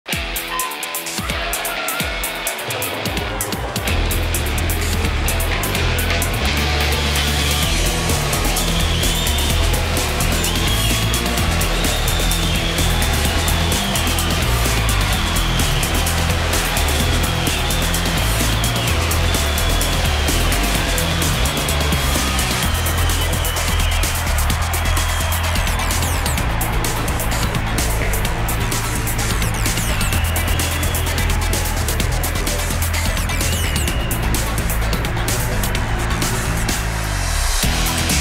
Blazing fast, 20 years apart!
One of the most entertaining Olympic races, the 50m freestyle sprint is incredible to watch, even 20 years apart!